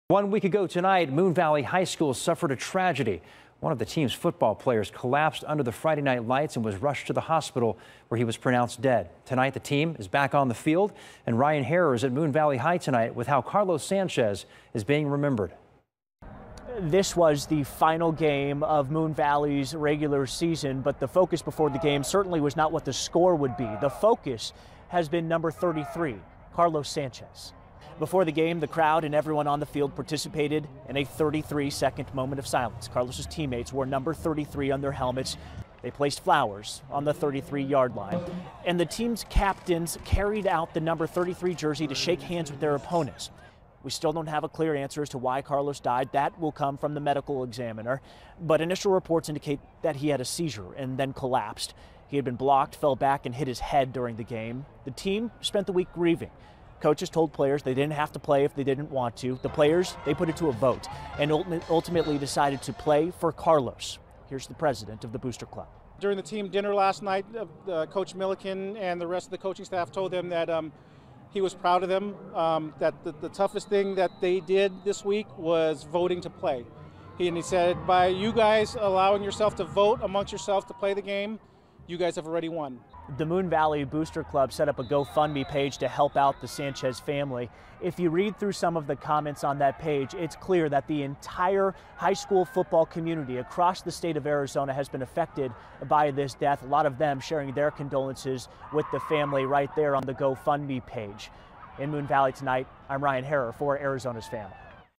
High school teams across Arizona to honor Carlos Sanchez
It&rsquo;s been a week since Carlos Sanchez, 16, collapsed and died while playing in a Friday night football game for Moon Valley High School. The Moon Valley Rockets will take the field in their final game of the season tonight at home against Peoria.&nbsp;(October 27, 2017)